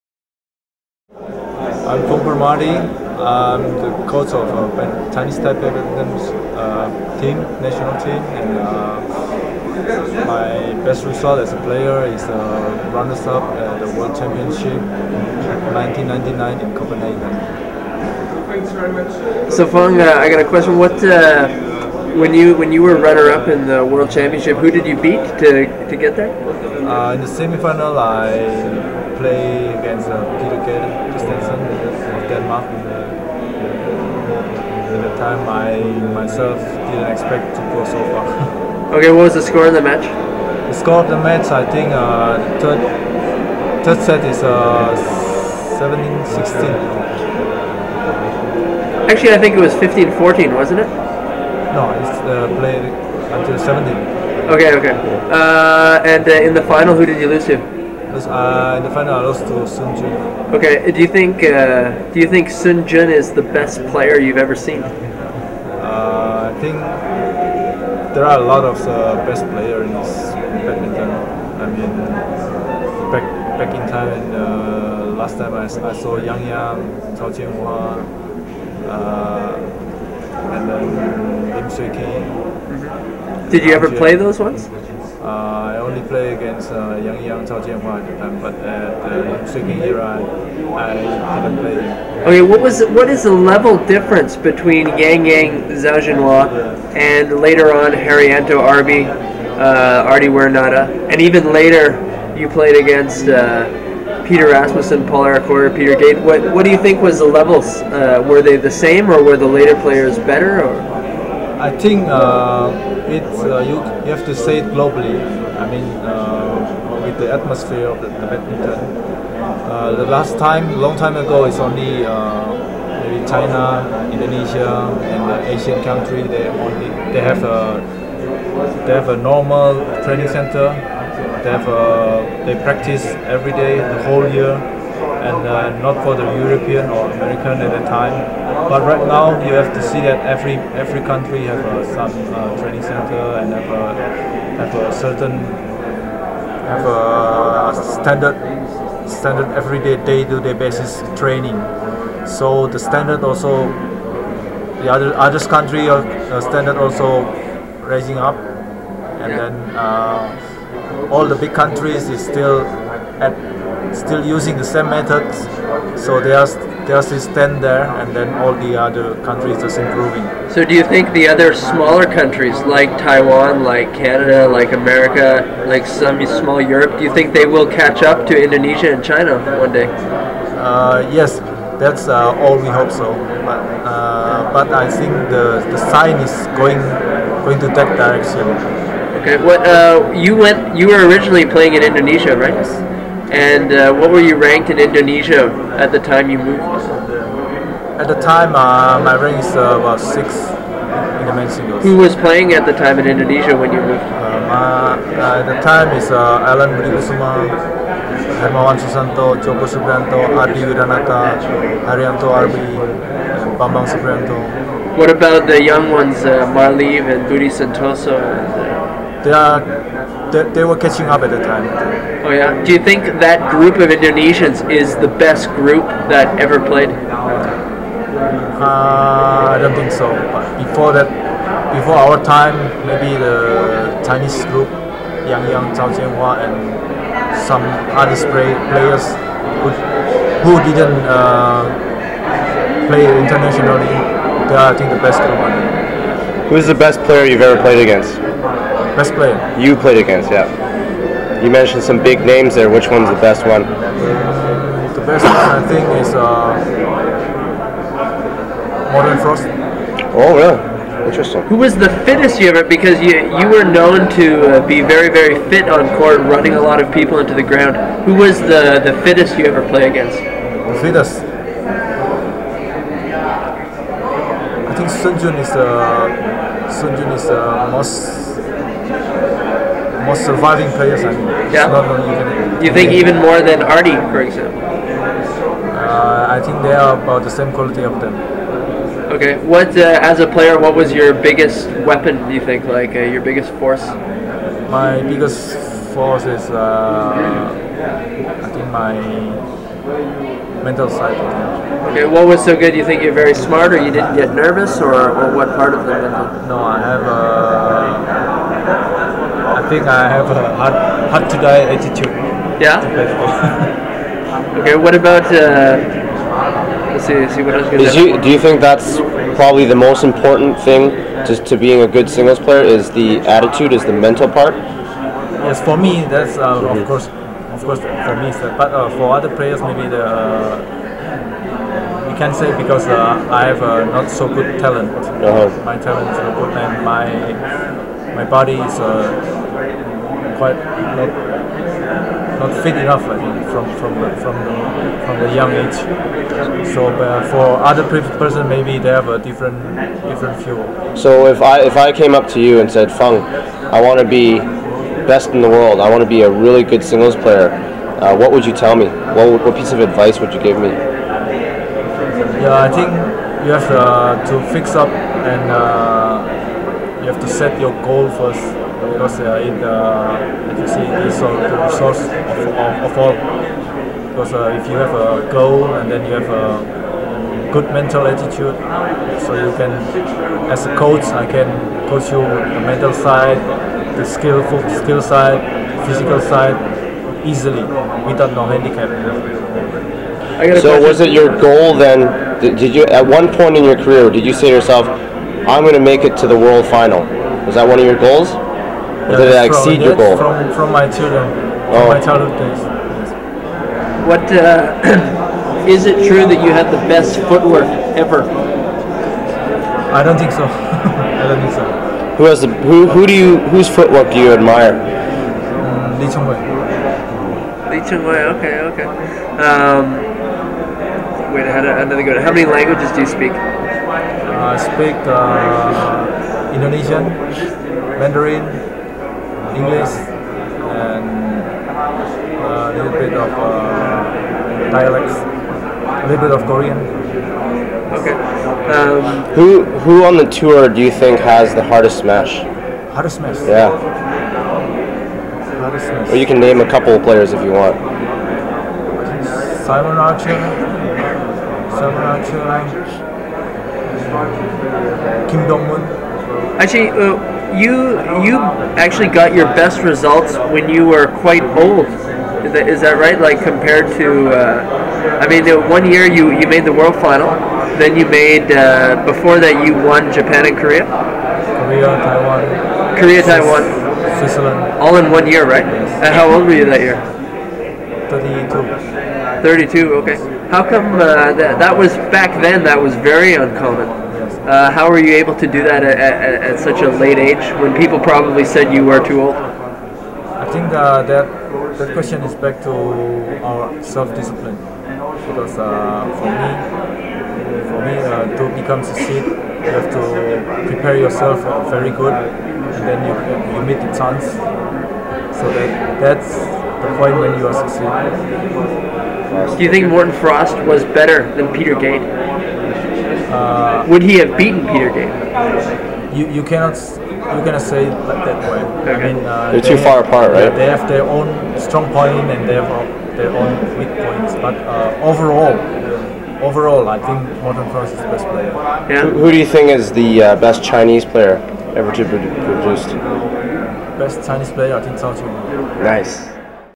Interview with Fung Permadi
Fung Permadi is the Chinese Taipei Team Coach.  He is also the 1999 World Championships Mens Singles Silver Medalist.